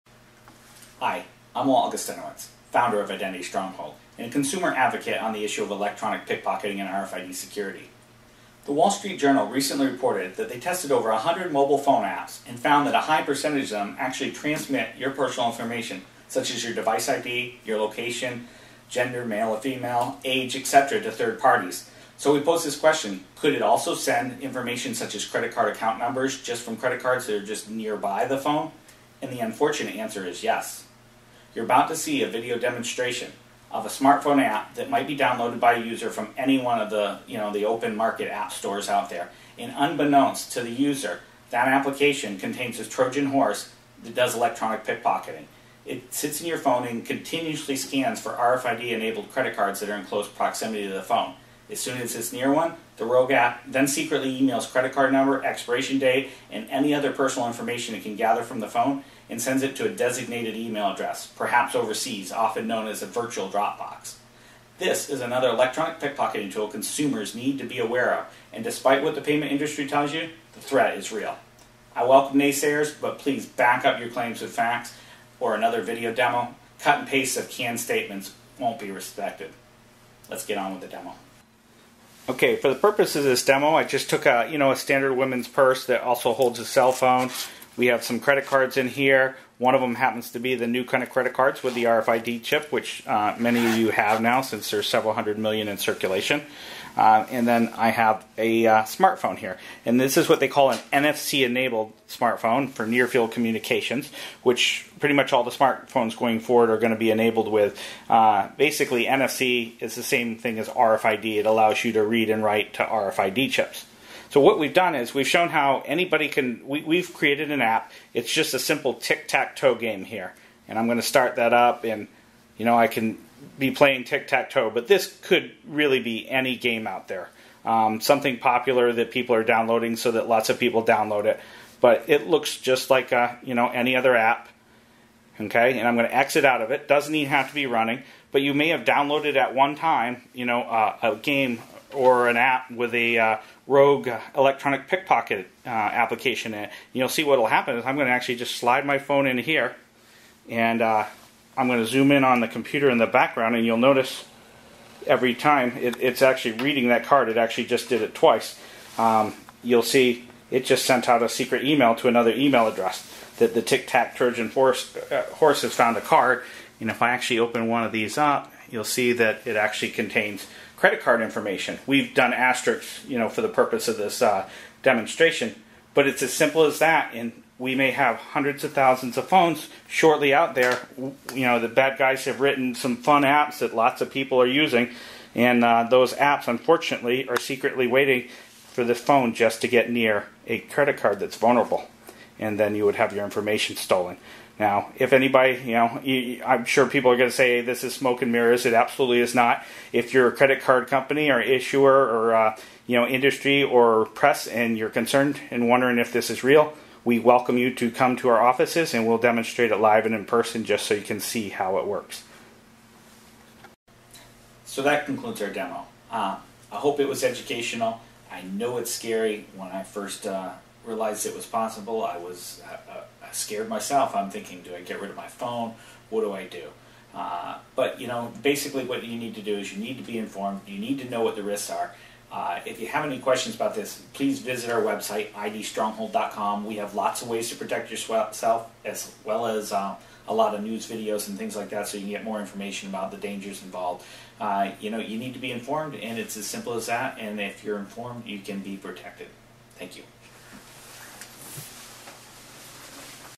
Trojan Horse Electronic Pickpocket Demo by Identity Stronghold
Identity Stronghold founder Walt Augustinowicz demonstrates how easily a rogue trojan horse program can be embedded in any app on an NFC enabled phone.